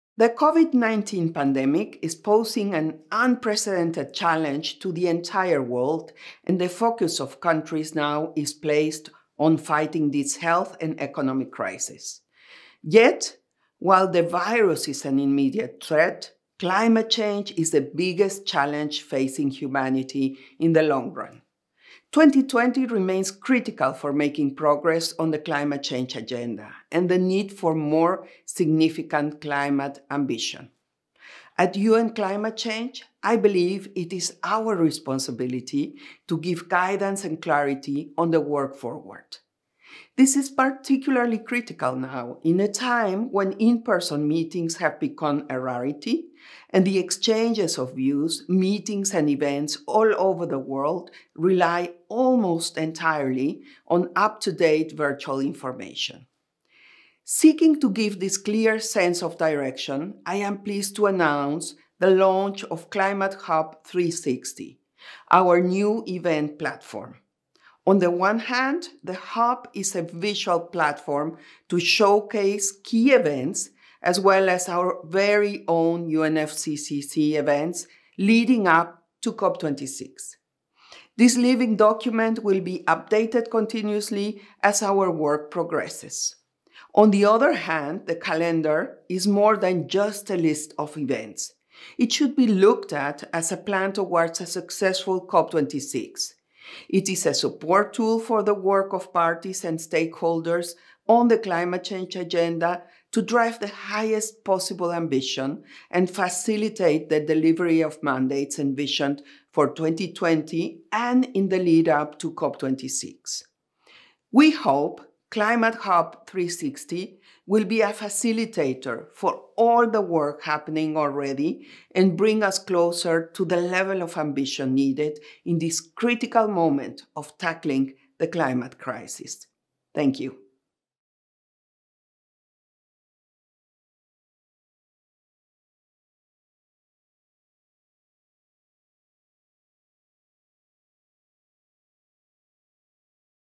Lancement de la plateforme Climate Hub 360
Patricia Espinosa, Secrétaire exécutive d'ONU Climat, annonçant le lancement de la plateforme d'événements Climate Hub 360.